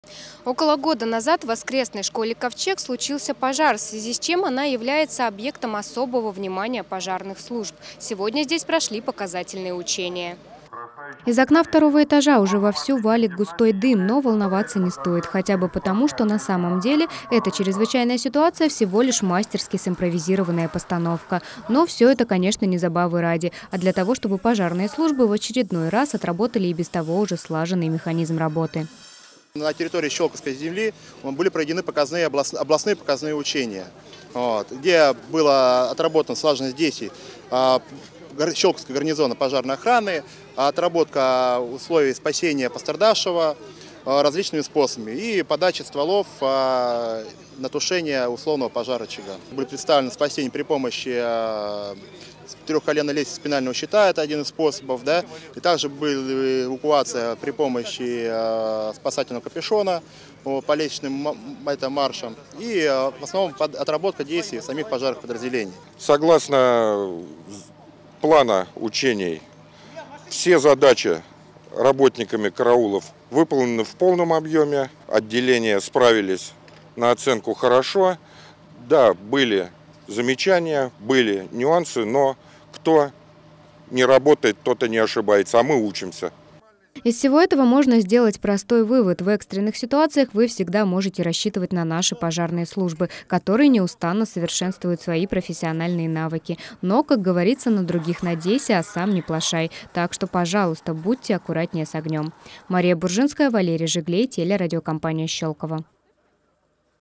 Пожарные учения в селе Душоново
Пожарные учения на объектах, которые требуют особо пристального внимания, постоянно проходят в городском округе Щёлково. На этот раз импровизированная спецоперация развернулась у воскресной школы «Ковчег» в селе Душоново.